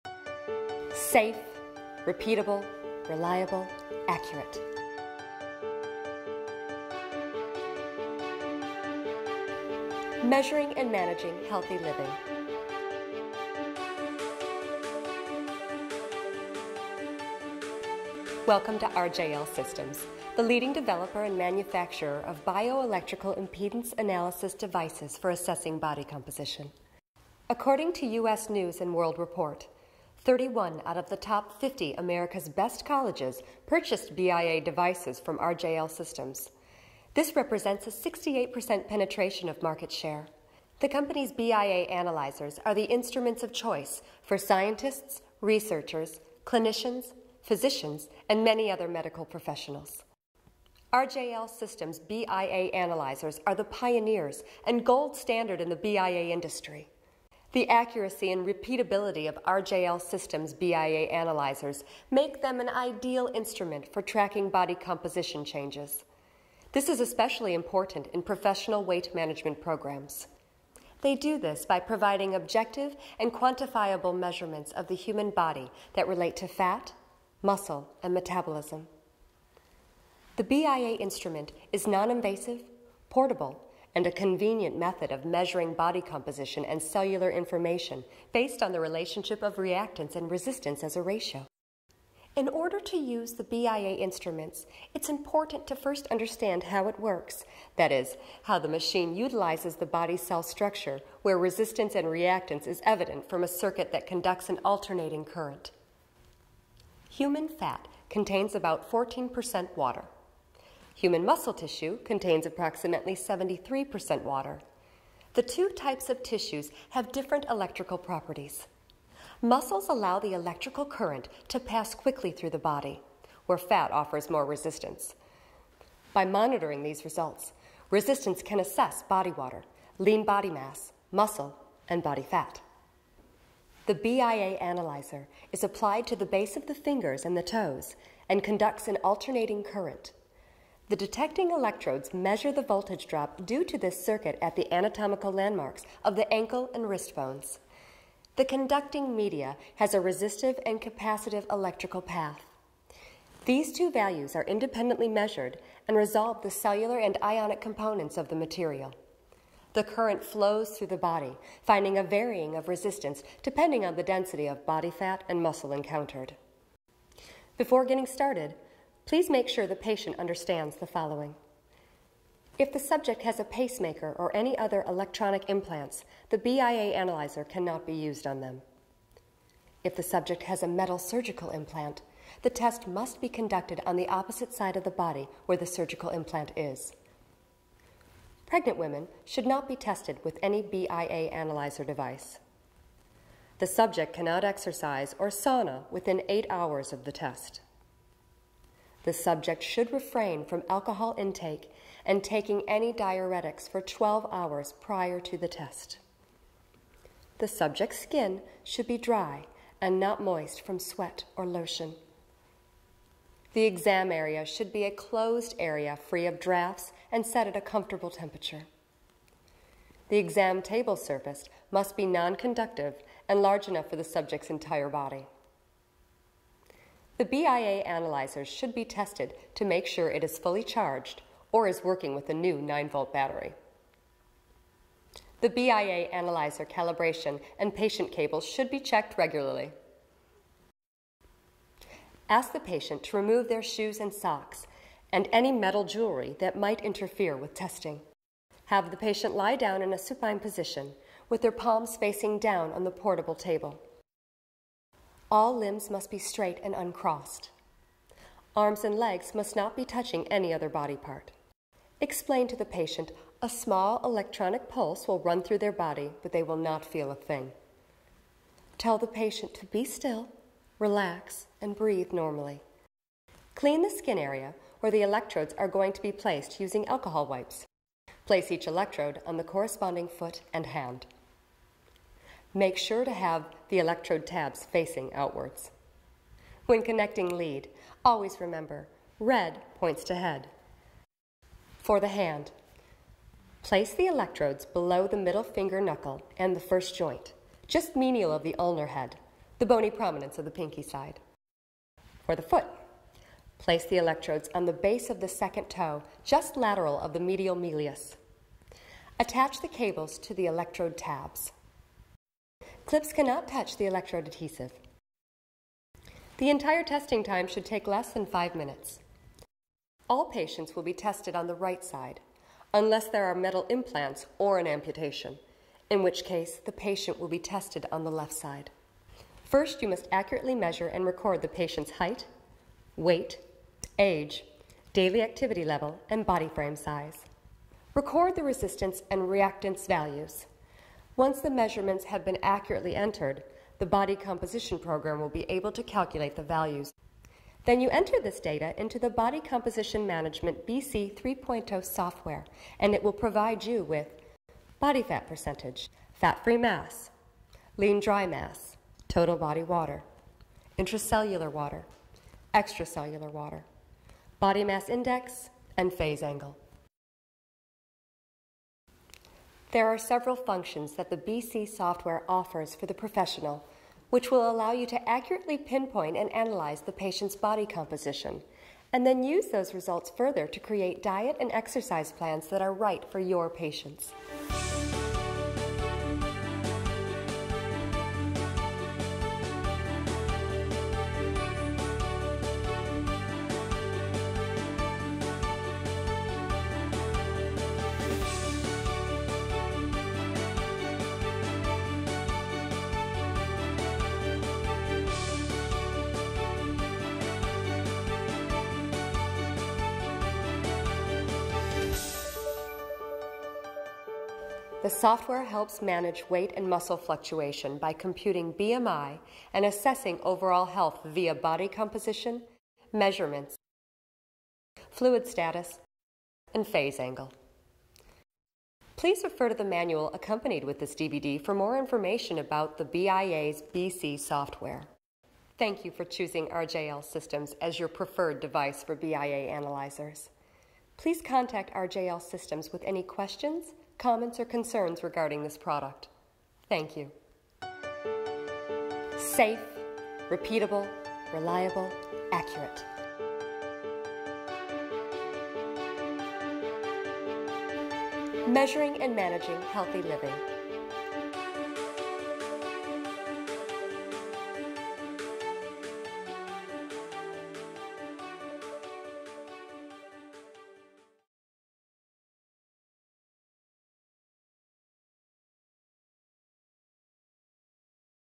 RJL Systems - Measuring & Managing Healthy Living - BIA Testing Procedure
RJL Systems is proud to present an in-depth tutorial video on the utilization of the RJL Quantum line of BIA devices. This comprehensive educational video provides details around our devices, software, testing, along with other important aspects of BIA testing. We hope you enjoy this video.